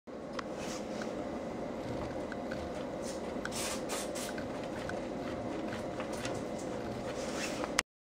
YASUKAWA SGM-02A312B AC servo motor (with a connecting cable) 動作確認
このＡＣサーボモータにＡＣサーボパックを接続し、パソコンソフト（Sigma-Win）のＪＯＧ運転にて正転、逆転の確認をする.